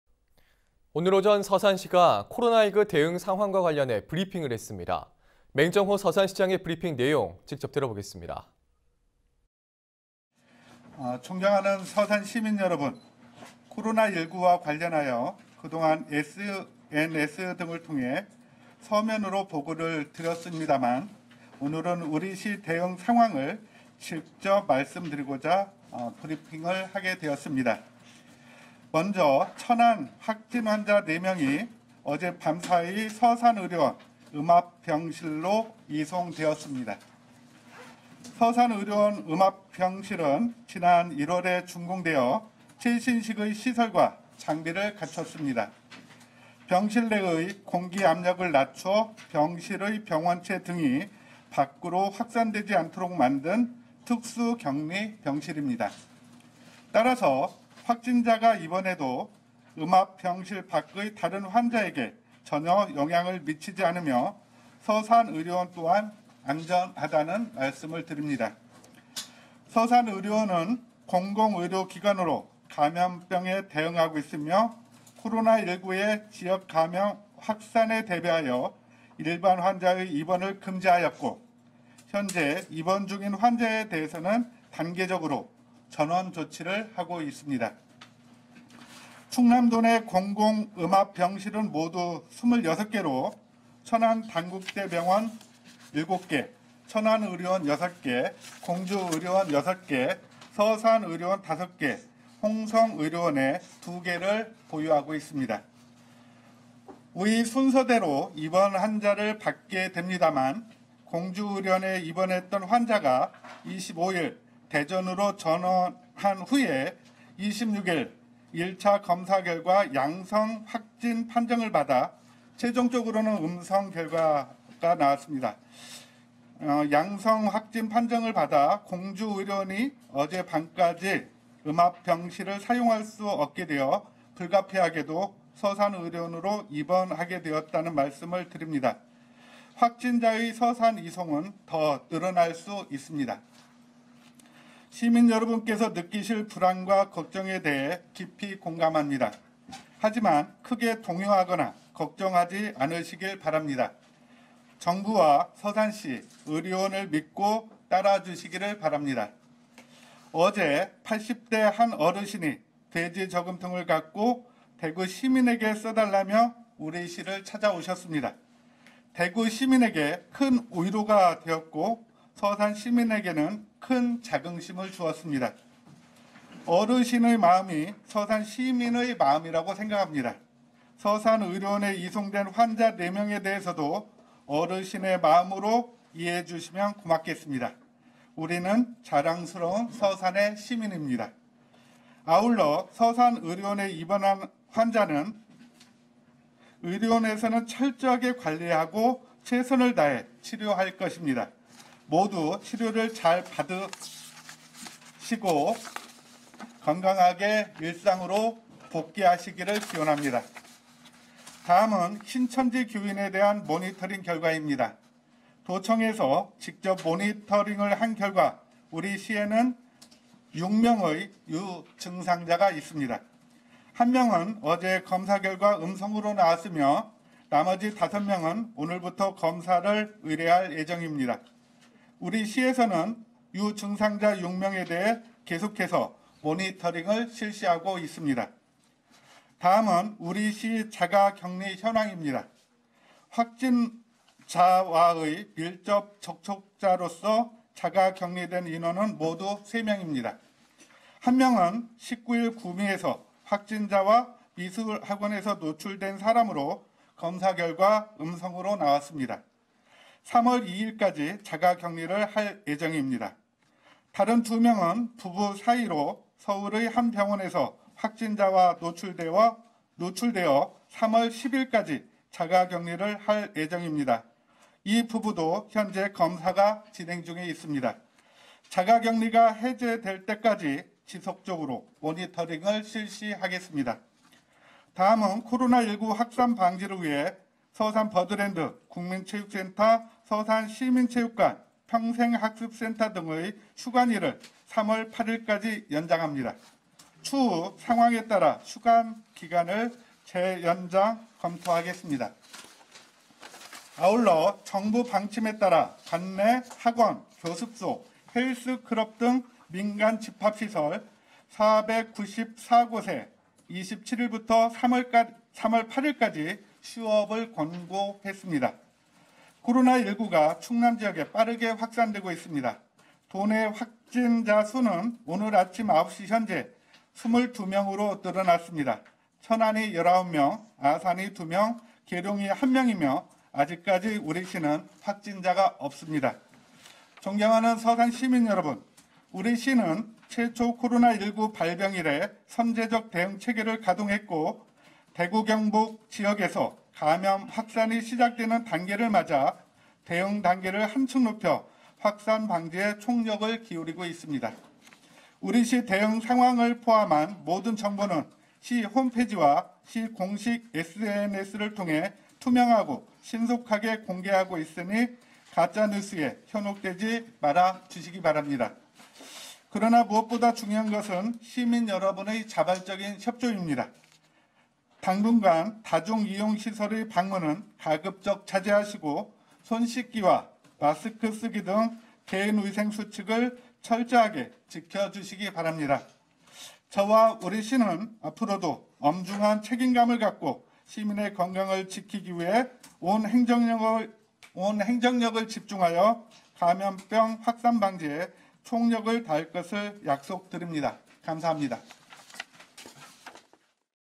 [오늘의 이슈] 맹정호 서산시장 기자회견
서산시가 코로나19 대응 상황과 관련해 브리핑을 했습니다. 맹정호 서산시장의 브리핑 내용 직접 들어보겠습니다.